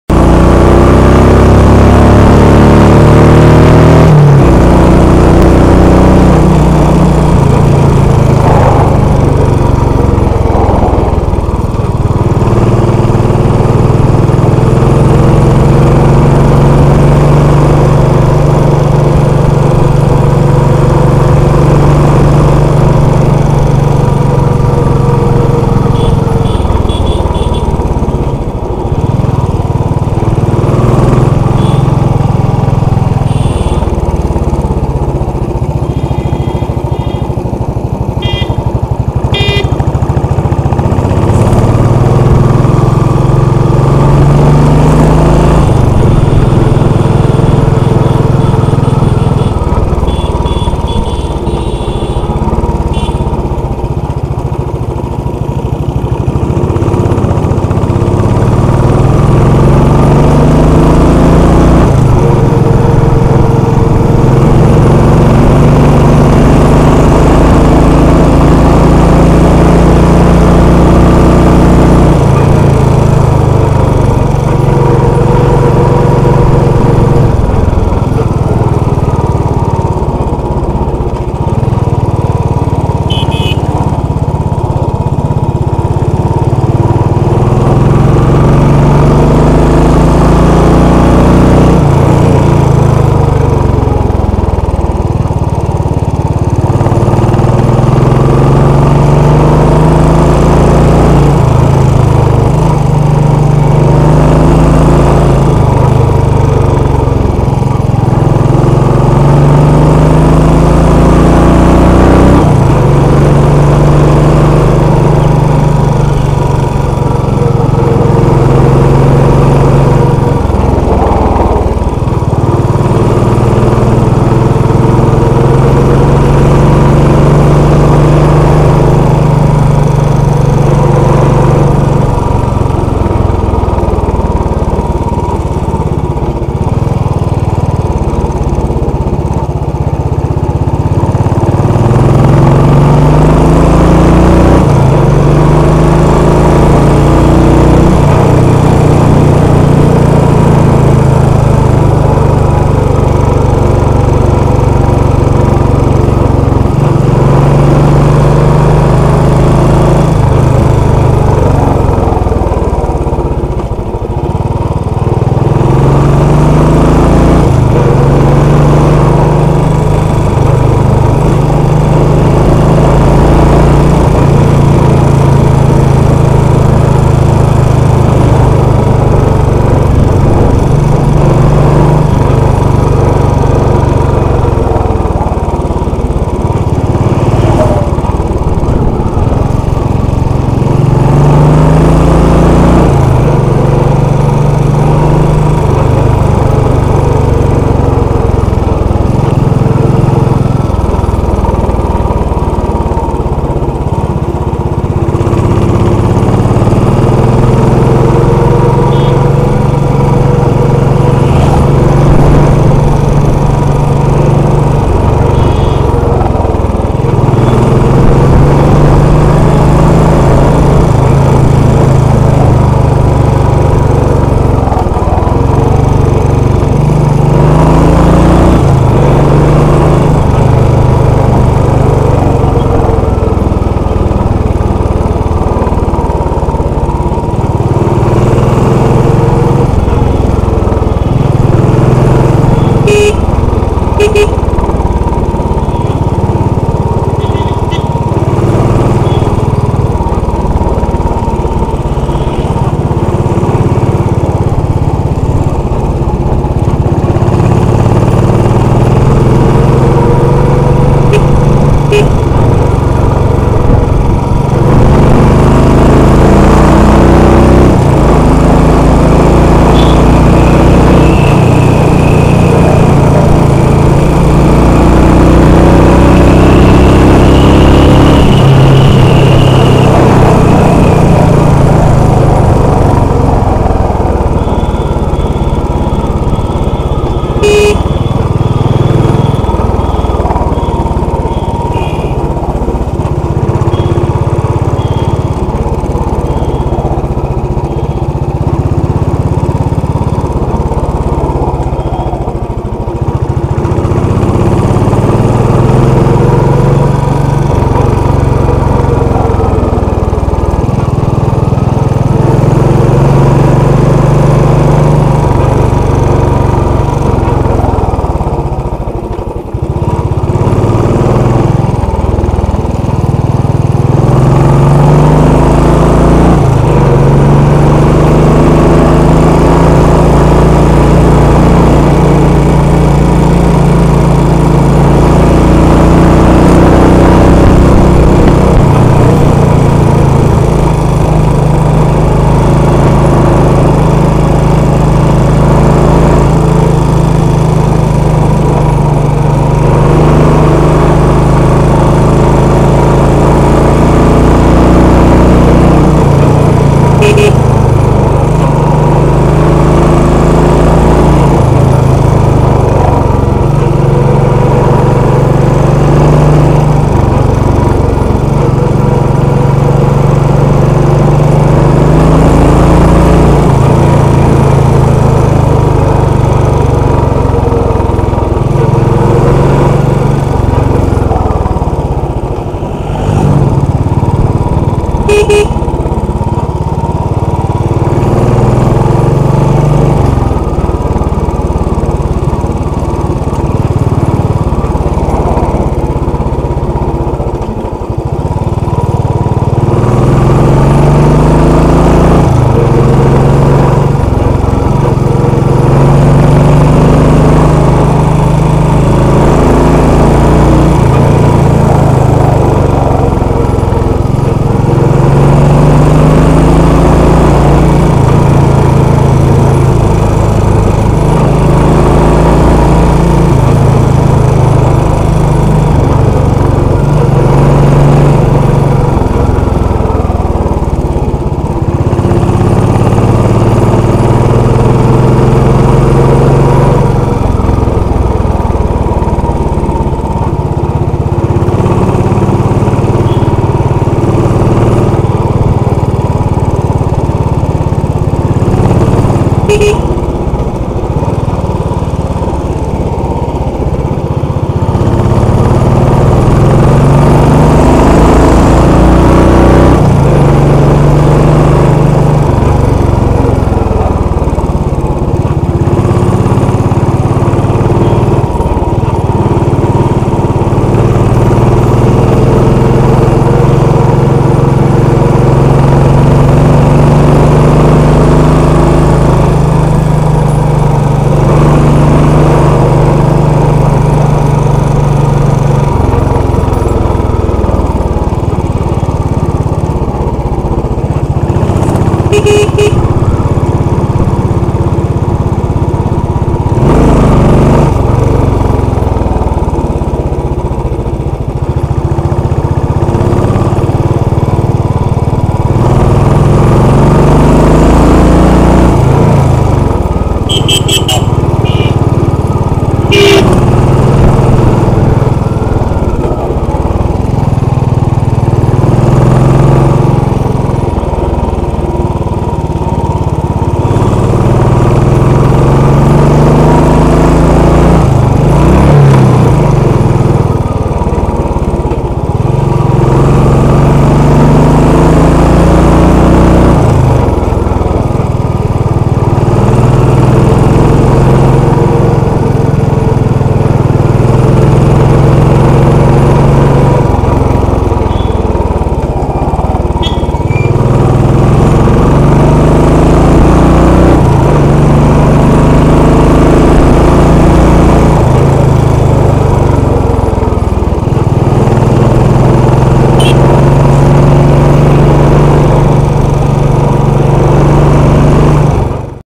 munnar downhill part 1.avi
riding down Munnar hills. Classic 500